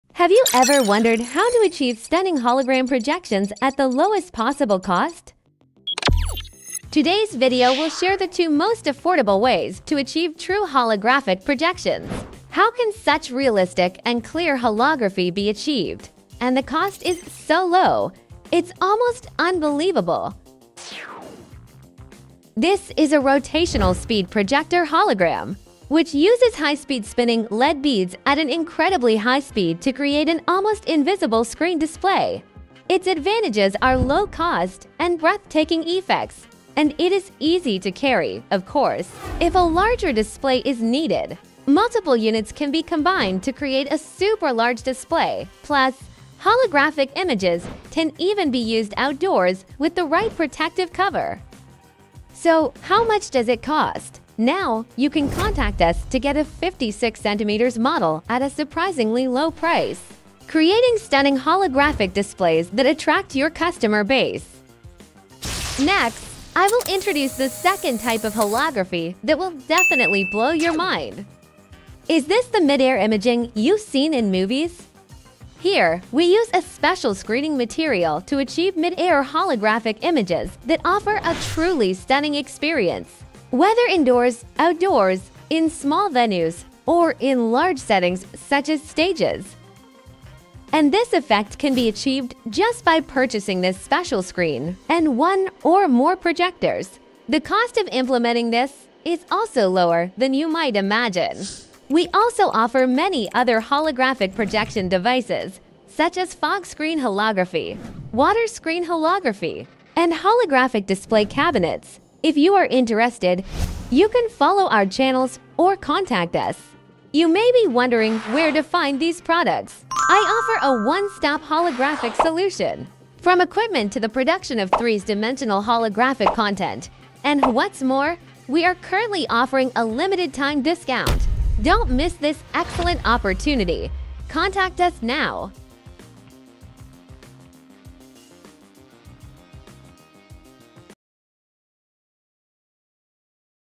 2025 update : How To Make a "Real" 3d Hologram Projector Or Fan Screen Display In The Cheapest Way?
2025 update : How To Make a "Real" 3d Hologram Projector Or Fan Screen Display In The Cheapest Way?
Discover how to create a captivating 3D hologram projector display for your next outdoor project or immersive experience, all on a budget!

Whether you’re planning an event, an outdoor installation, or an Immersive Enchanted Nightwalk experience, this video is packed with all the information you need to enhance your project with stunning 3D visuals without breaking the bank.

In this detailed tutorial, we’ll walk you through the steps to build a professional-quality “Real” 3D hologram using a hologram fan and a hologram big projection outdoor or indoor screen.

We’ll cover the essentials of choosing the right hologram fan and big hologram projector screen suited for outdoor conditions, setting up the system to withstand environmental factors, and achieving the best visual effects to mesmerize your audience.

Watch our comprehensive demonstration to see the hologram projector in action, learn troubleshooting tips for common outdoor challenges, and gain insights into making your display as effective as possible.

⏰ Video Duration: 00:02:24

Timestamps:
0:00 - Introduction to hologram projector
0:27 - Budget Hologram Setup Explained

1:15 - Affordable Large-Scale Hologram Screen
1:50 - Explore Various Hologram Techniques